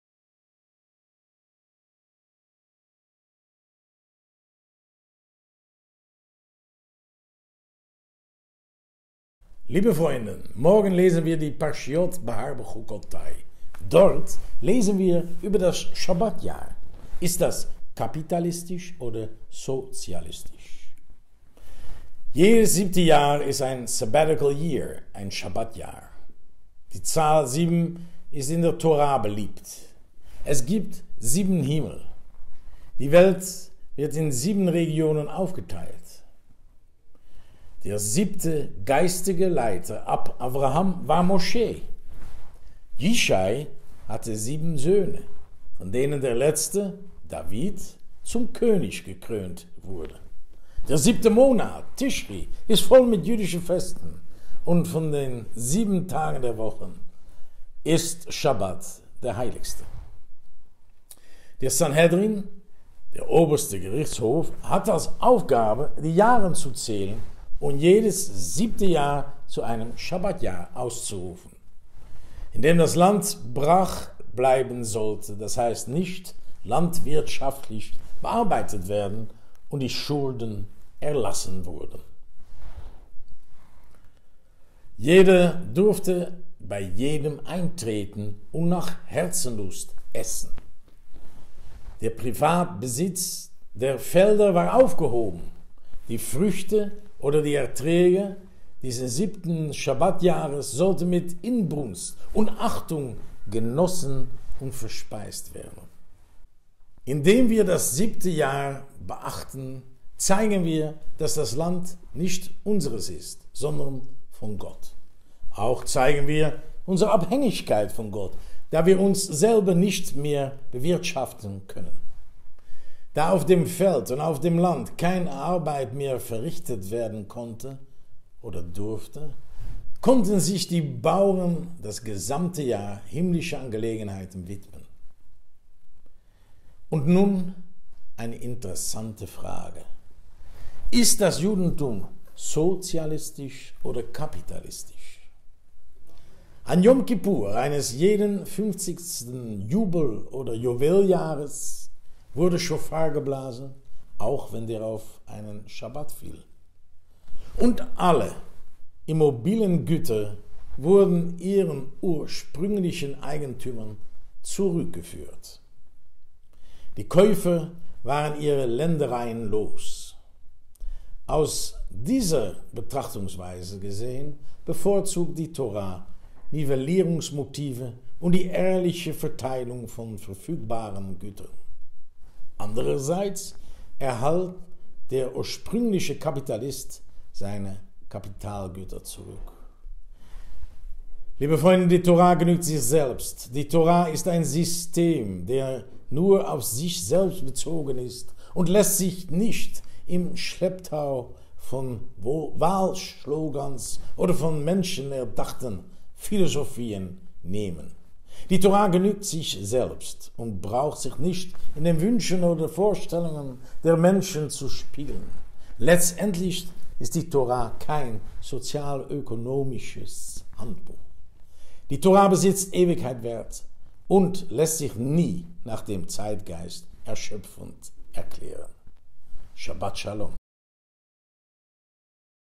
Gedanken von Oberrabbiner Raphael Evers über die Parascha Behar-Bechukotai
Oberrabbiner Raphael Evers erklärt die Bedeutung der Zahl sieben und nimmt dabei Bezug auf die Paraschat HaSchawua "Behar-Bechukotai".